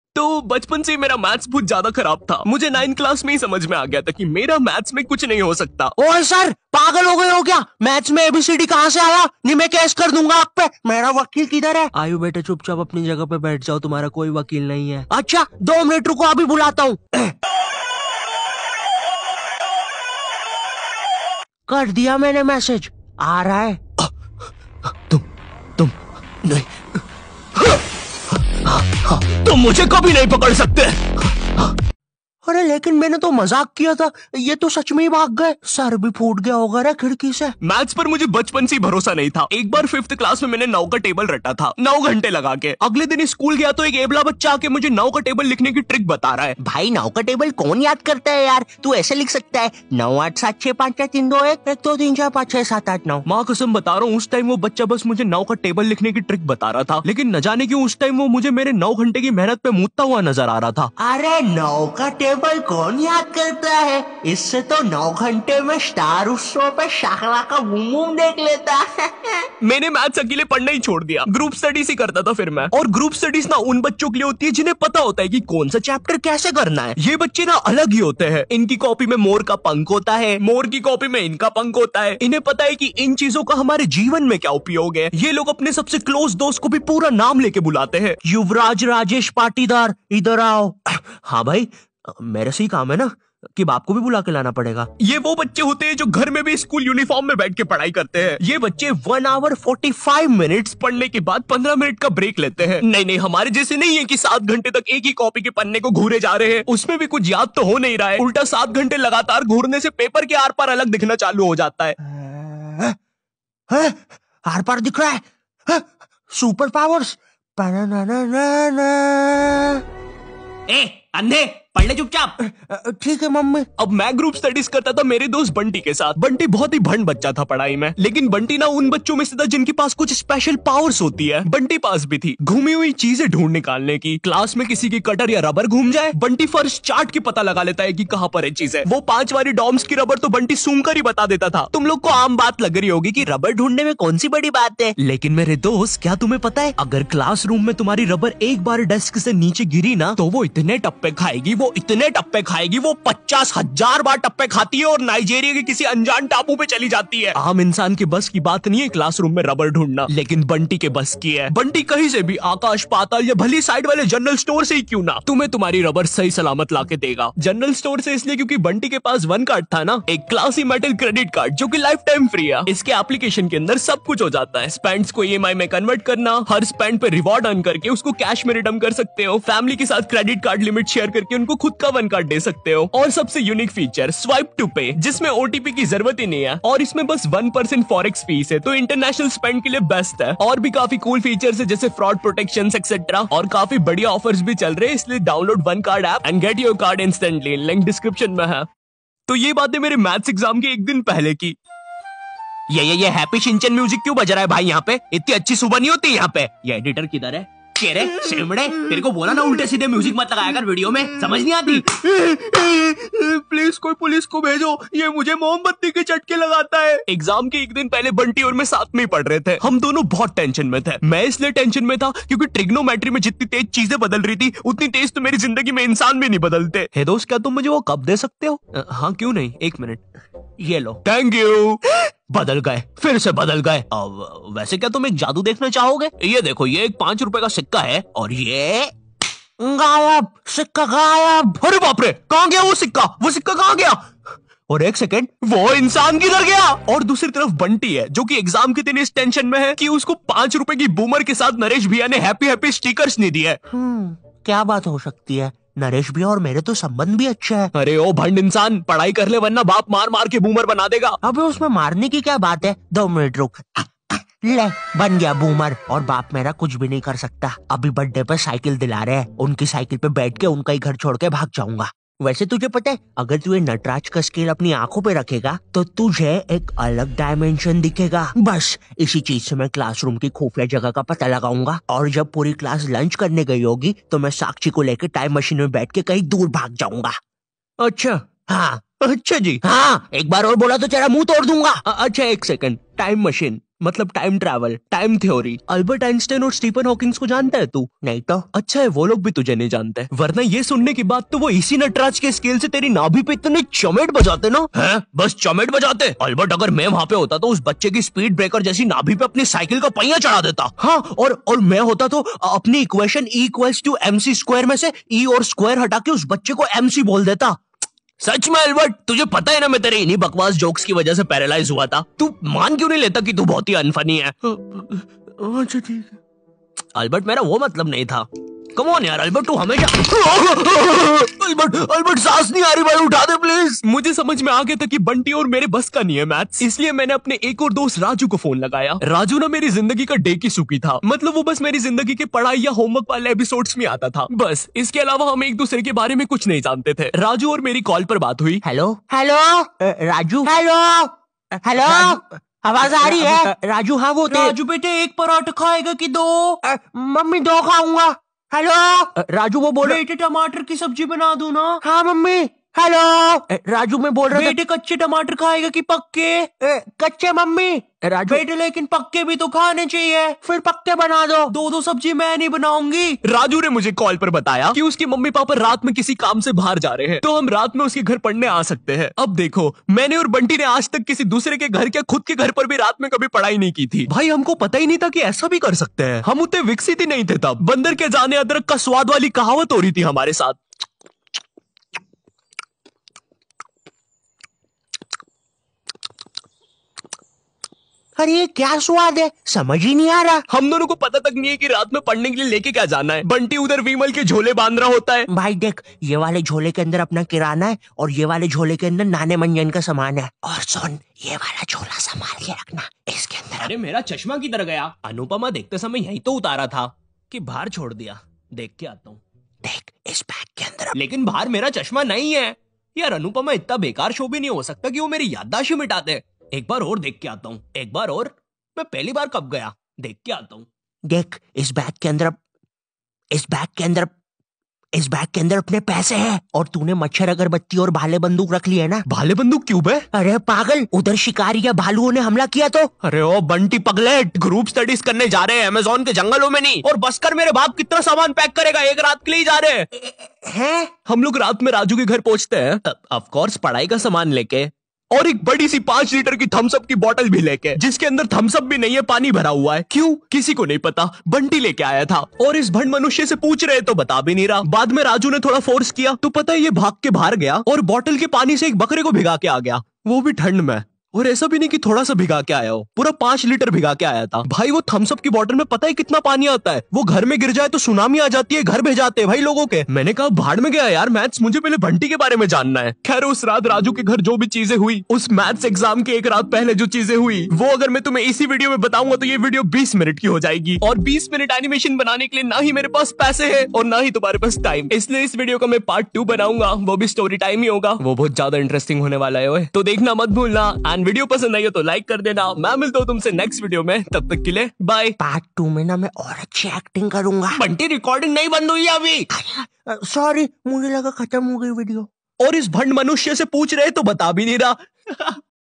Group studies in exam time
Commerce Parne walo ke hall jo ky aap iss video me dekhy gy